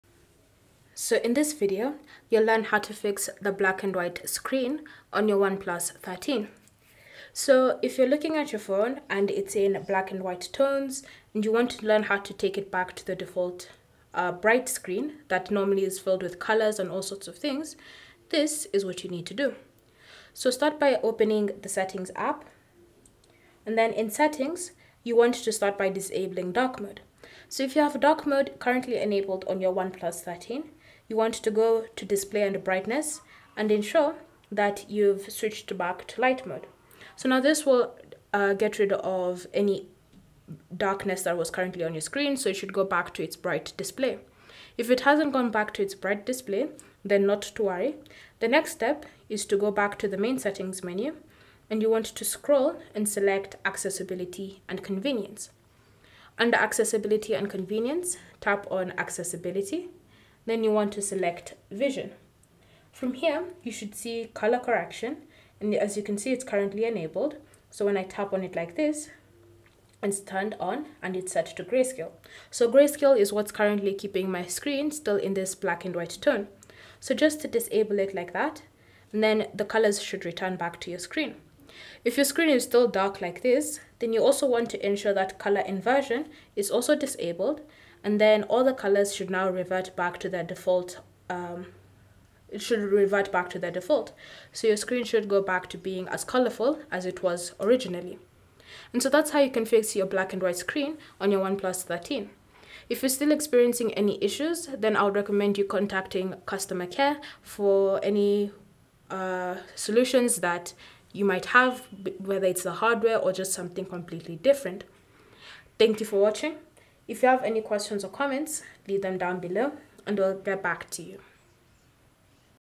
How To FIX Black & White Screen On OnePlus 13
How To Repair Black & White Screen On OnePlus 13
Is your OnePlus 13 stuck with a black-and-white screen? Don’t worry! In this video, I’ll walk you through simple troubleshooting steps to fix the black-and-white screen issue and restore vibrant colors to your display. Whether it’s a software glitch or a setting you accidentally enabled, we’ve got you covered.

How To mend Black & White Screen On OnePlus 13
If the dark mode is on, Start by disabling it on display & brightness
Select light mode. If the black & white doesn't disappear,
Then go back to the main setting menu & select
Tap on accessibility & conveniences
Tap on Accessibility
Navigate to vision tab
Select color correction
Disable the color correction toggle
Also, ensure color inversion is disabled

Cell Phone Tripod Adapter